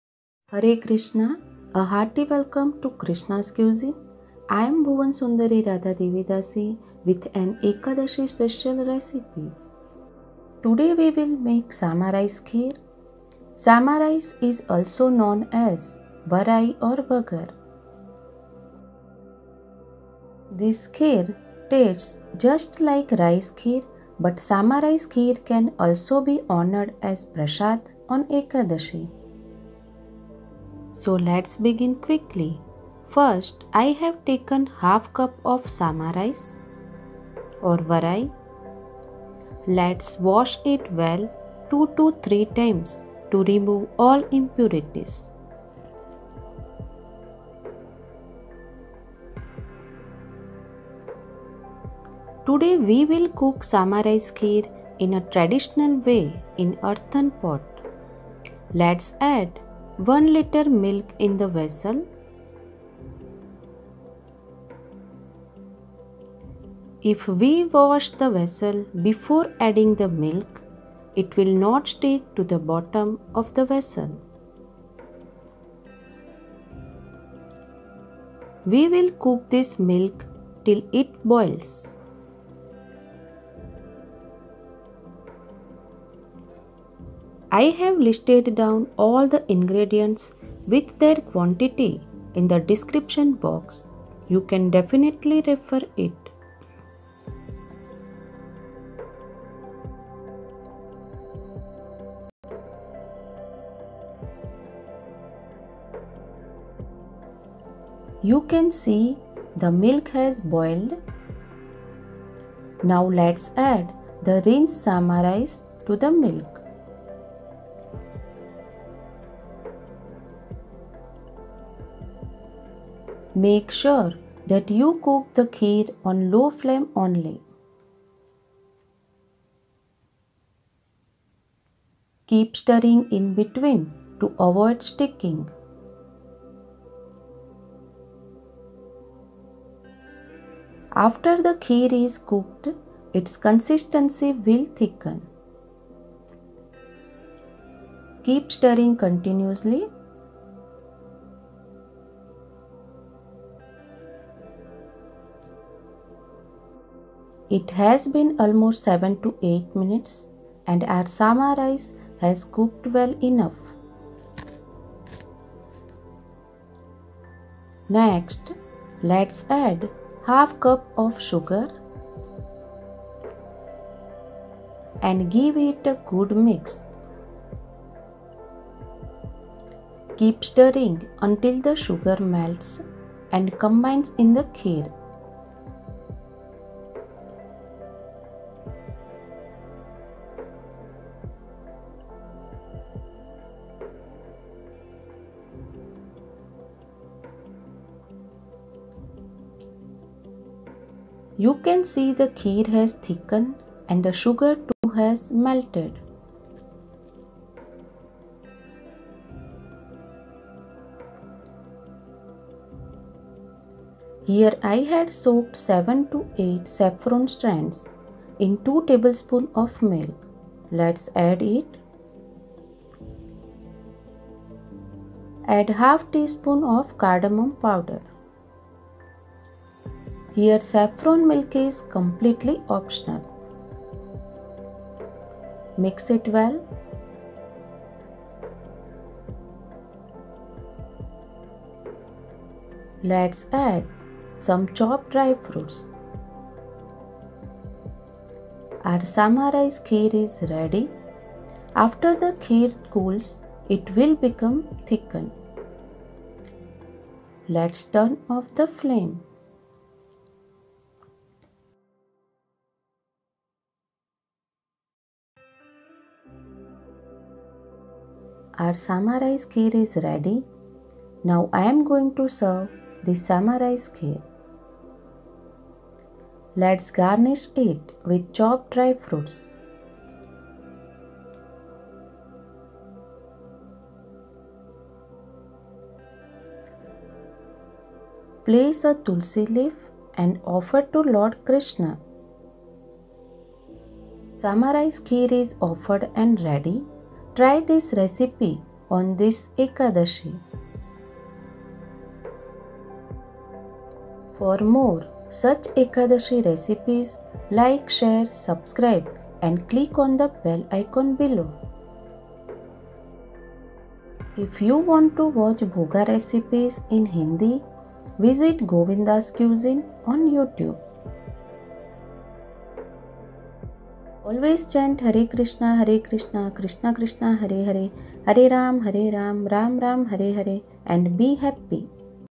Ekadashi Special Varai Kheer || Sama Rice Kheer || Krishna's Cuisine
Hare krishna!!

Sama rice kheer is a delicious kheer recipe made up of varai and milk with a crunch of nuts. It goes very well during any fastings and it tastes similar to rice kheer.
For more recipes, search for Krishna's Cuisine in youtube and scroll down for all..

Servings: 5

Ingredients:

1/2 cup sama rice/ varai
1 litre cup milk
1/2 cup sugar
1/2 tsp cardomom powder
7 to 8 saffron strands
2 tbsp milk to soak saffron strands
1 tbsp chopped almonds and pistachios for garnishing